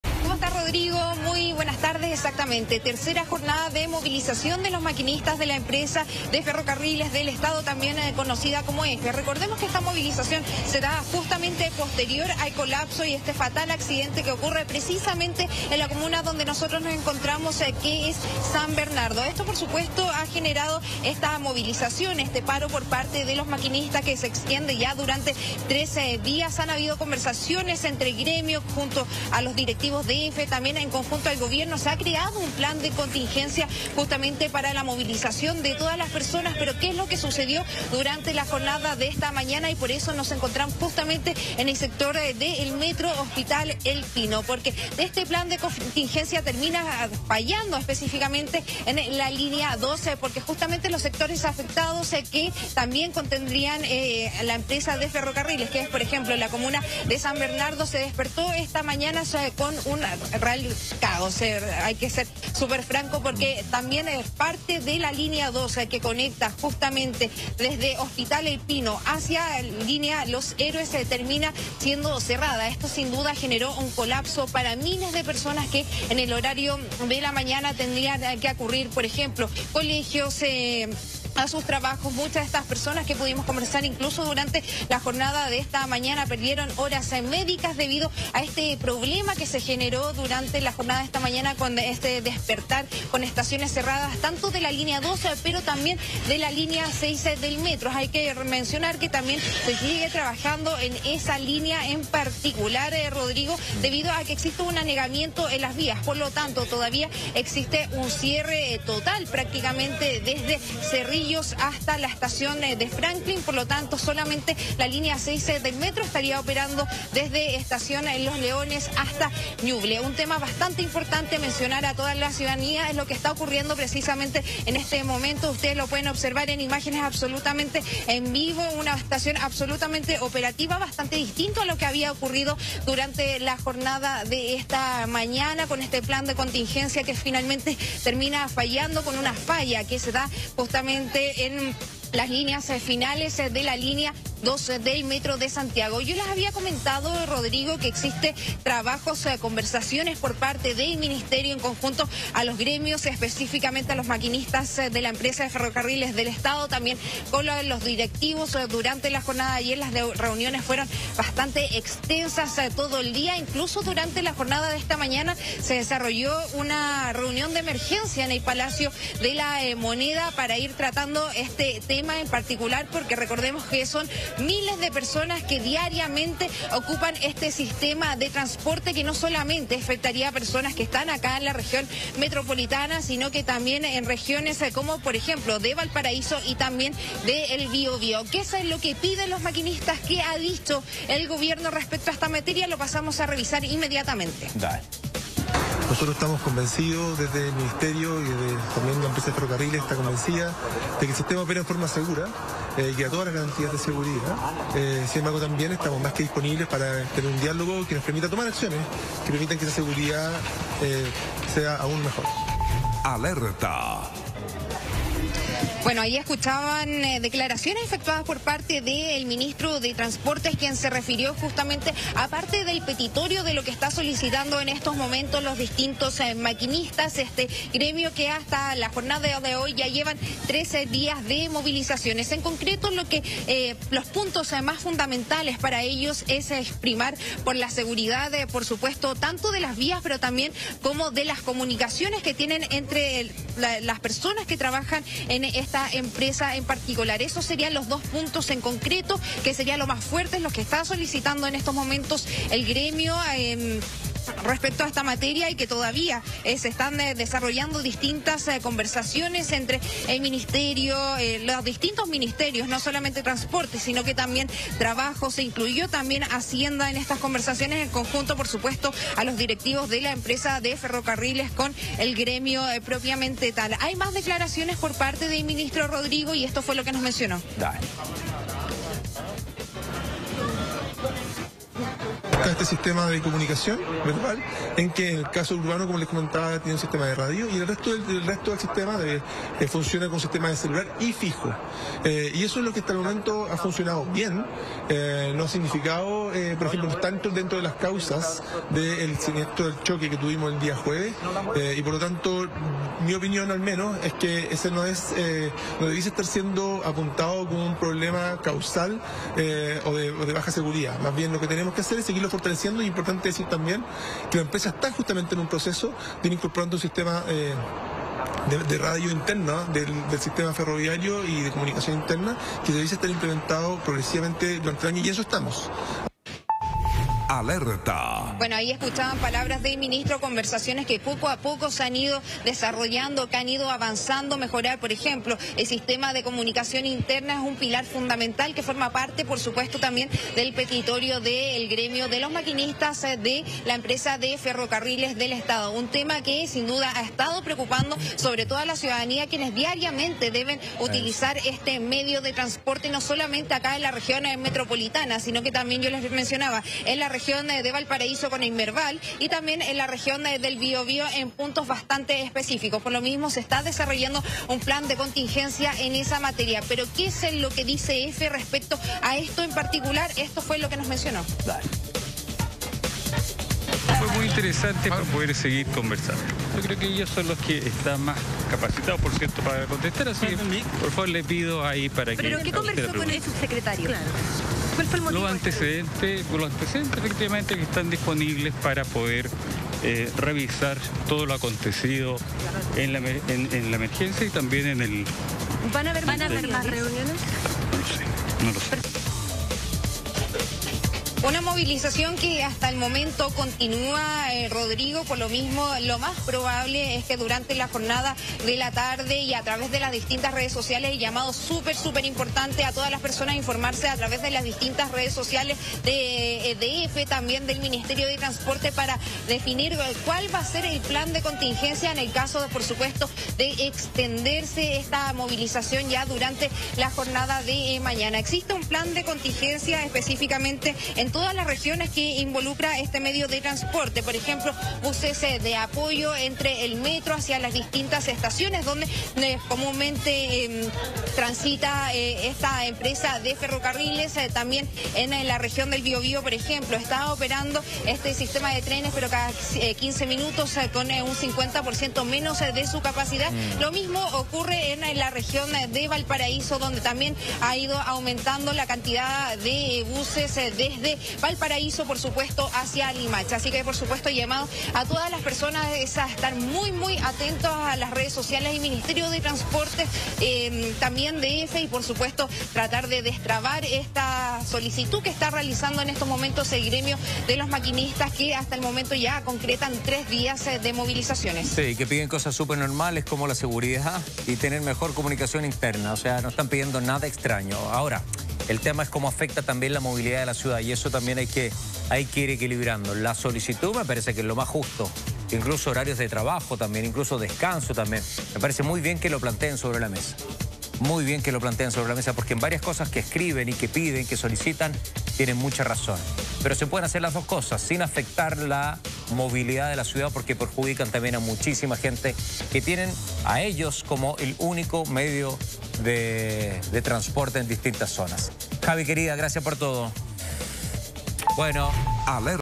Caos en el sector sur: Paro de trenes e inundación en el metro complica la movilidad
El sector sur de Santiago tuvo un caótico despertar con dos de las principales vías de movilidad cerradas.